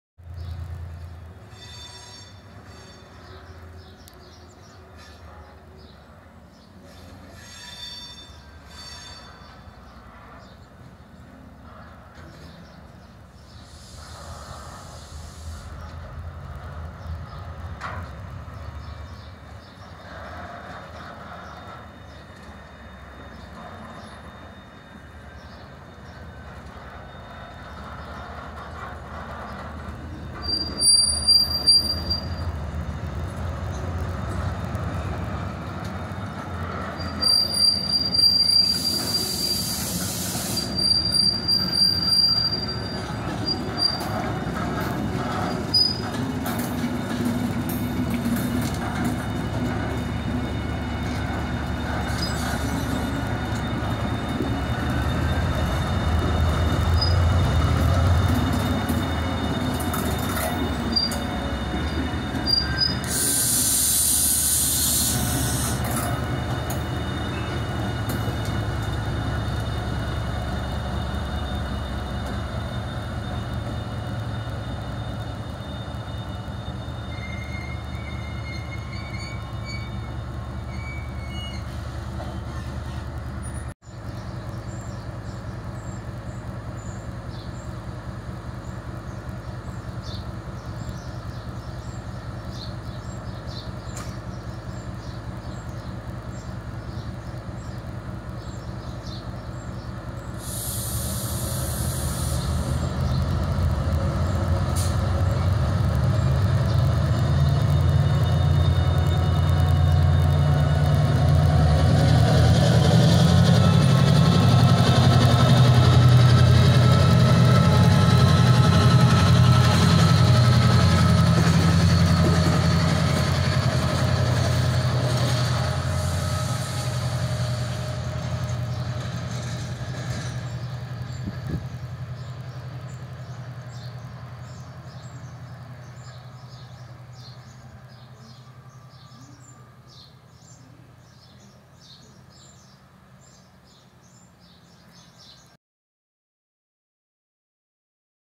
68001 & 68016 head back to Crewe.
5th June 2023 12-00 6M63 Bridgwater to Crewe. 68001, 68016.